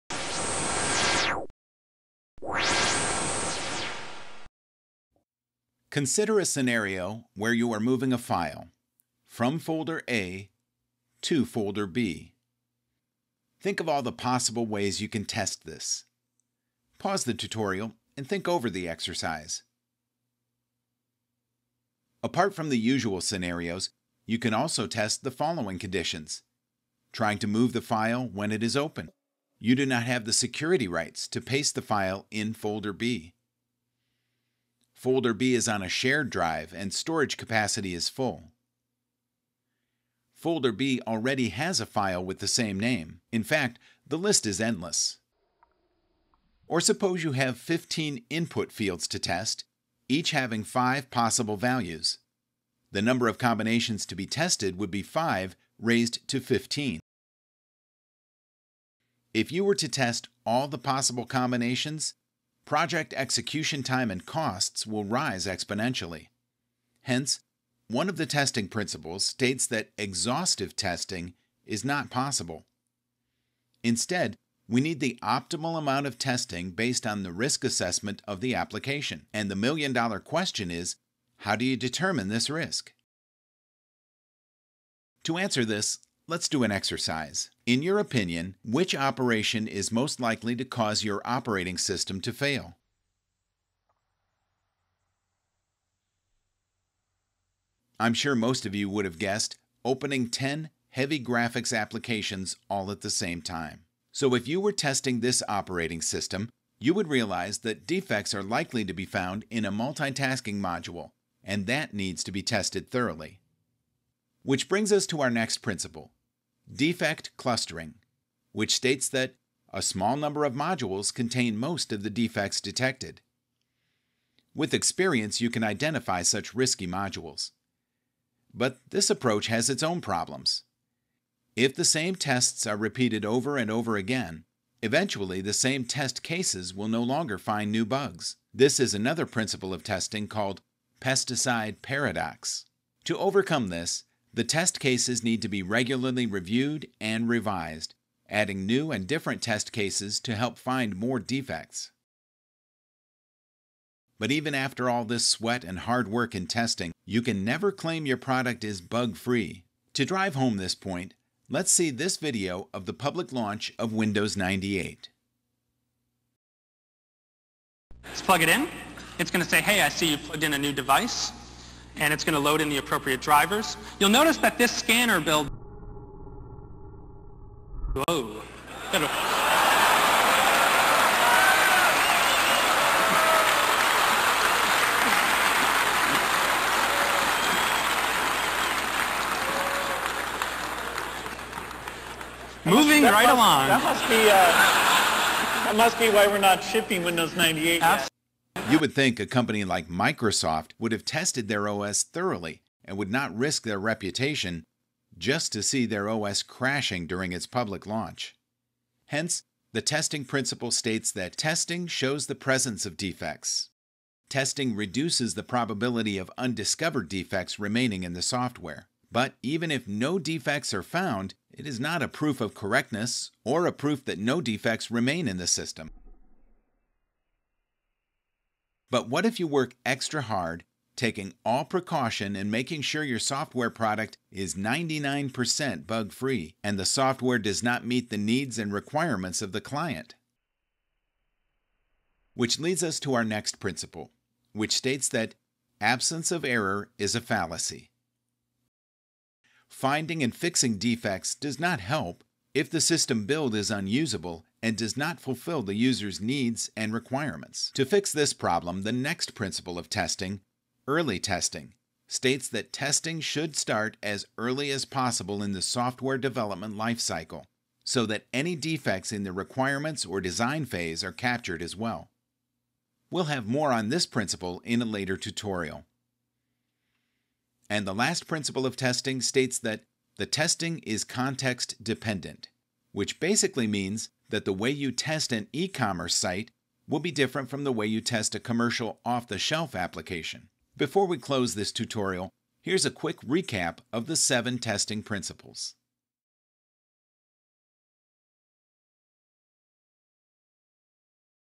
Seven Testing Principles: Software Testing
This Testing tutorial for beginners explains 7 types of Testing in Software Testing. The principles help in testing methodologies and techniques.

Principles of Software Testing in brief –
Principle 1 Testing shows presence of defects
Principle 2 Exhaustive testing is impossible
Principle 3 Early Testing
Principle 4 Defect Clustering
Principle 5 Pesticide Paradox
Principle 6 Testing is context dependent
Principle 7 Absence of errors - fallacy

Enjoy our free tutorial on our YouTube channel and our website.

READ this as a book on Amazon by viewing

Sincerely,
Guru99